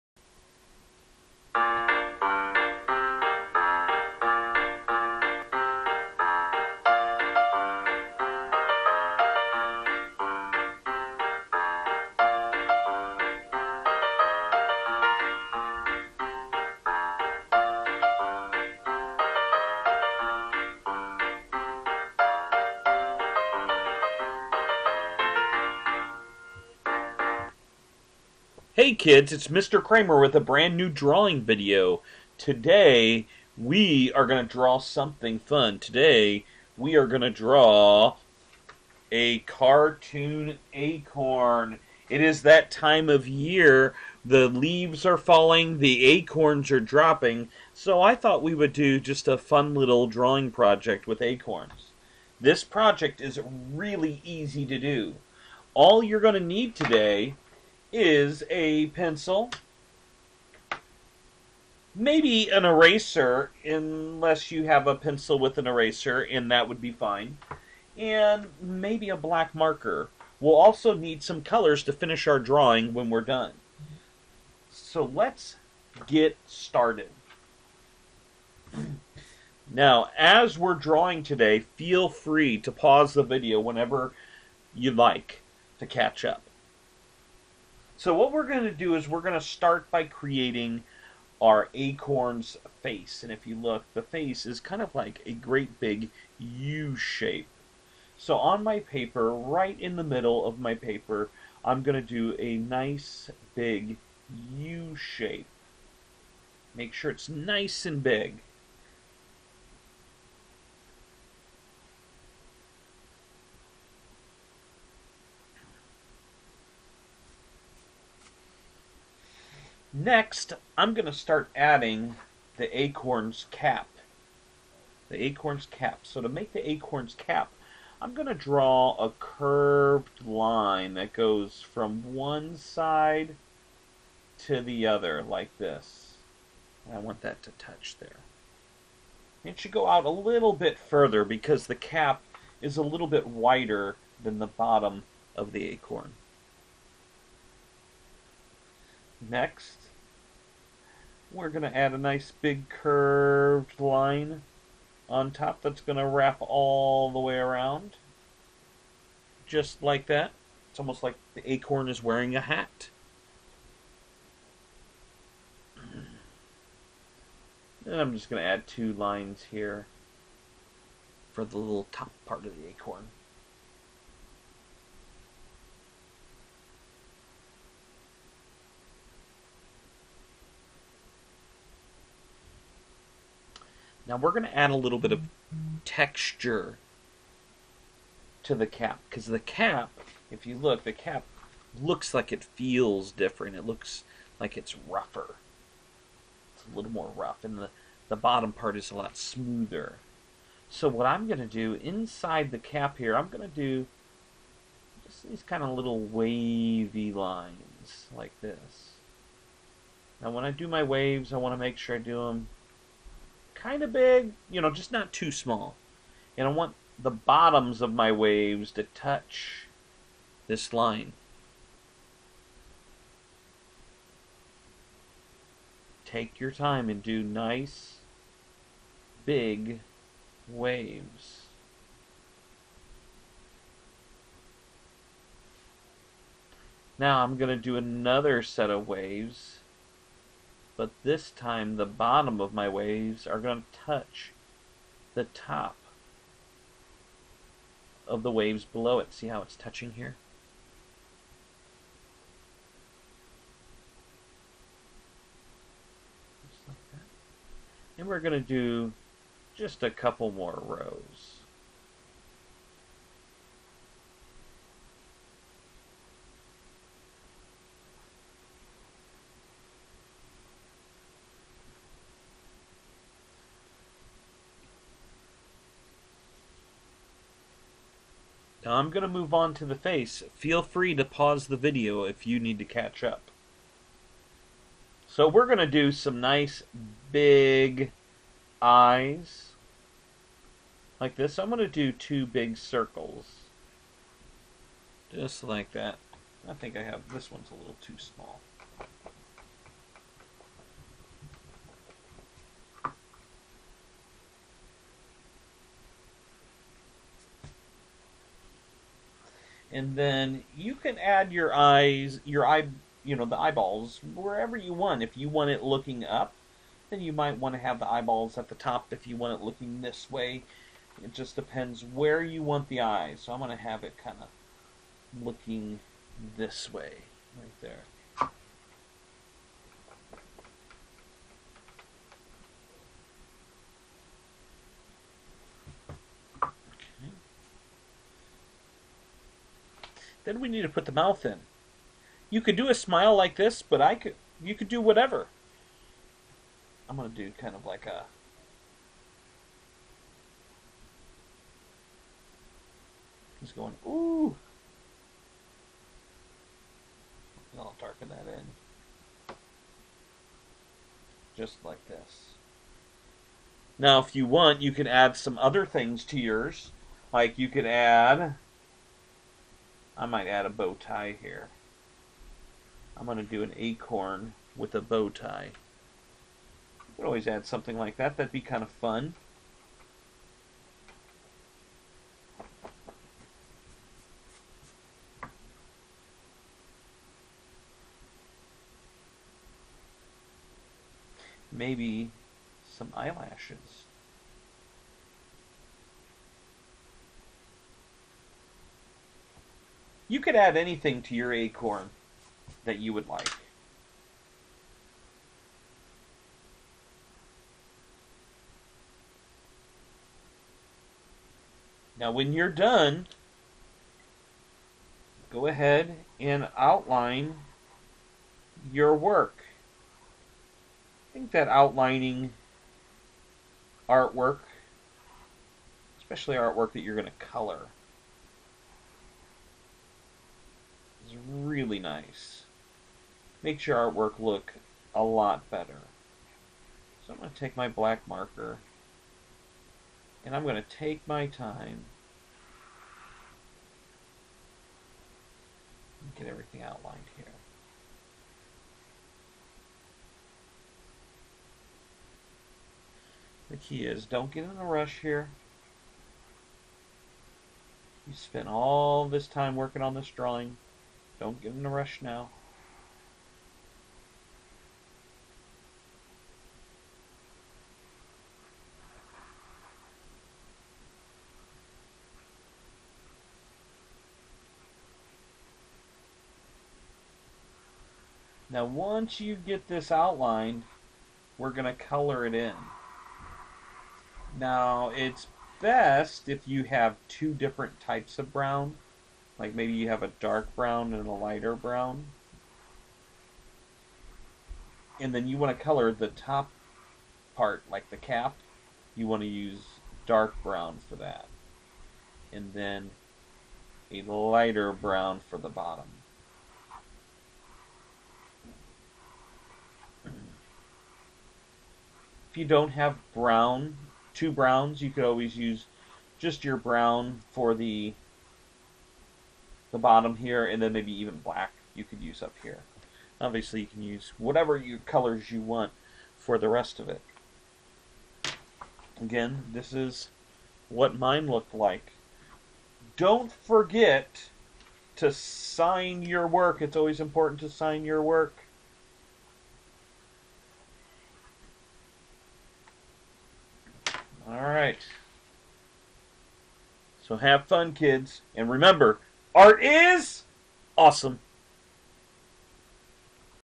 Mr Dan the Art Man_Acorn drawing: Pre-k through 1st Grade
Here is a fun drawing video of an Acorn. Have fun and remember that art is AWESOME!!1

Recommended ages pre-k-1st.